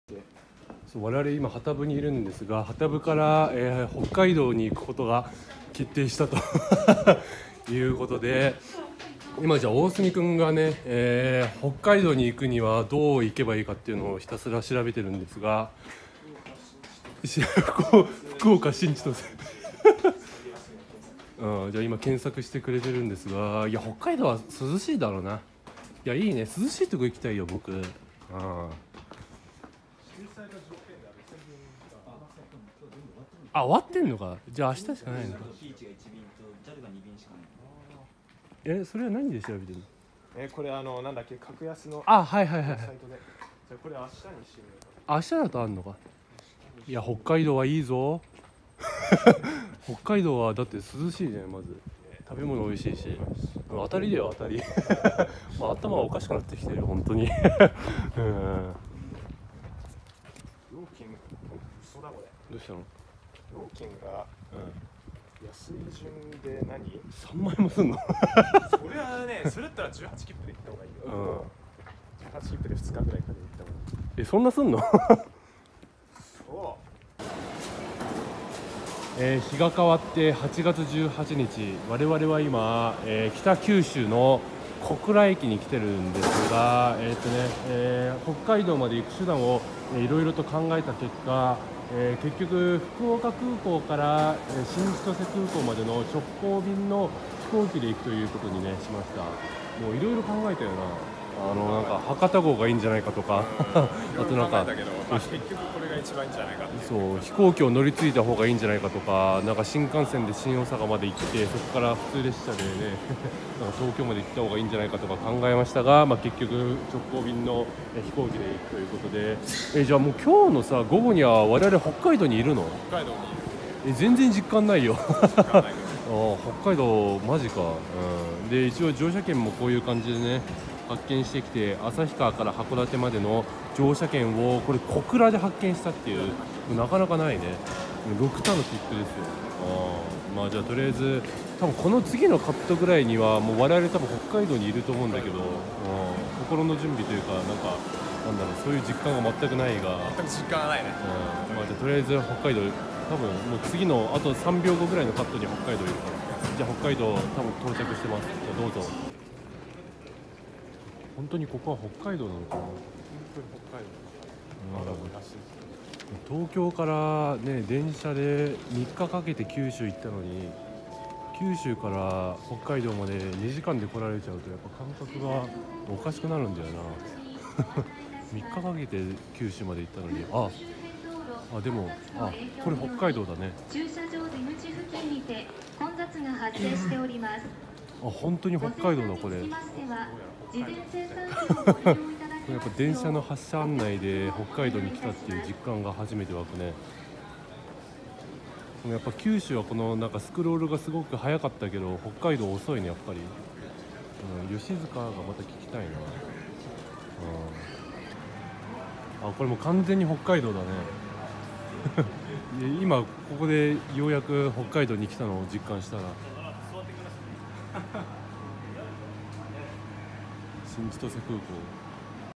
【下関→北海道】我々の旅の様子。
2回目の選択後、下関→北海道に行くことが決定してしまい正気を失いかけてるがみ。
鬼畜在来線乗り潰しの旅のルール説明を1日目の生放送で行ったので、初見の方はアーカイブを見てください。
旅の最中は毎日生放送中！
旅の様子をリアルタイムで更新！